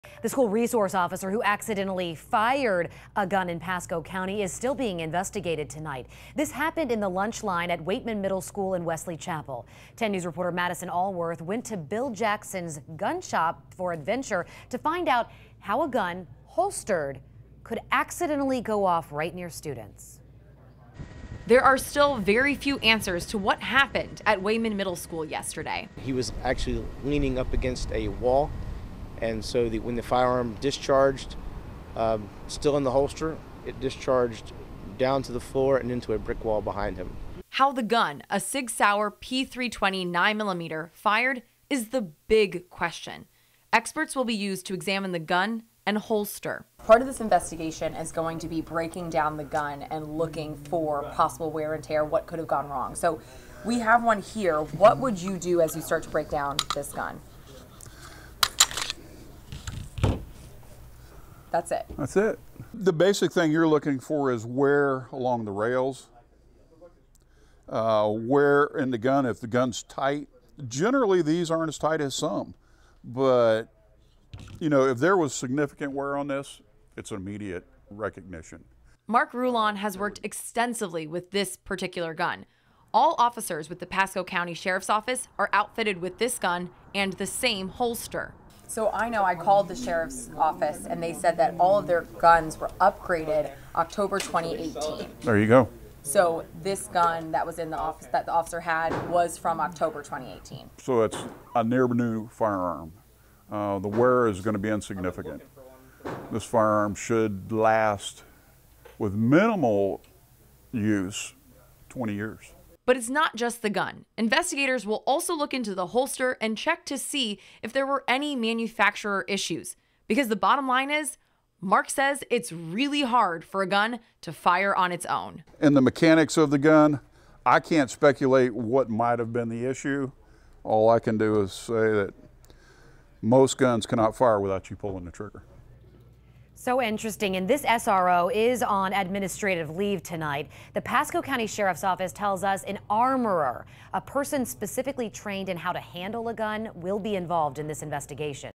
Investigation into Pasco School Resource Officer whose gun discharged in a middle school cafeteria c
There are still very few answers to what happened at Weighman Middle School yesterday.
The school resource officer accidentally discharged his gun while in the school cafeteria line.
There is no word how long the investigation will take, but how the gun, a Sig Sauer P320 9 millimeter, fired, is the big question. Experts will be used to examine the gun and holster.